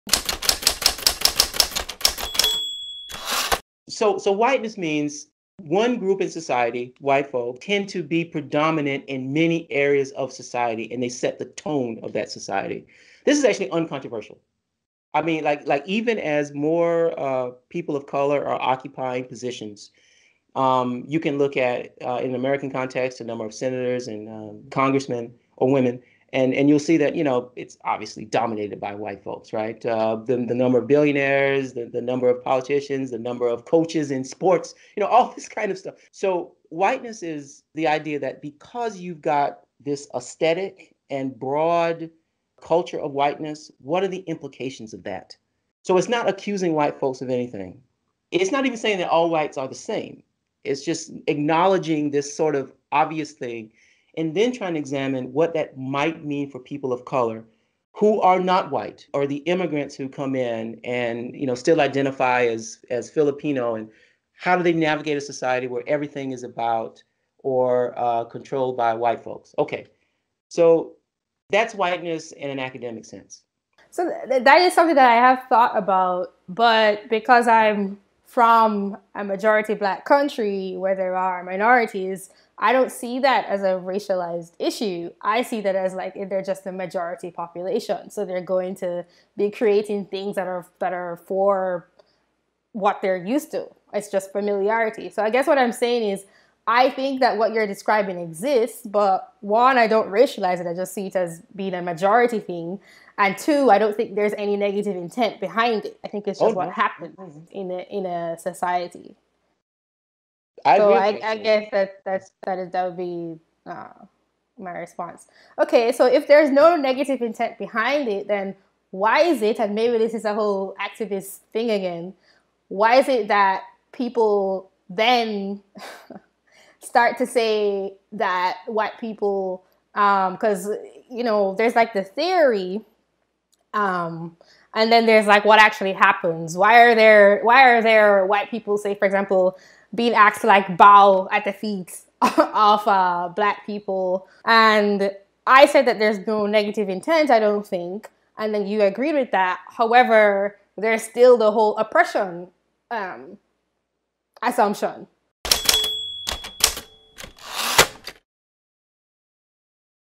JTOL CLIPS 🎬 "Whiteness" in an academic vs activist sense
Full video - Rod Graham - A Proponent of Critical Race Theory, Words are Violence, and More!

Also available as a podcast. Search for Just Thinking Out Loud in your favorite podcast directory.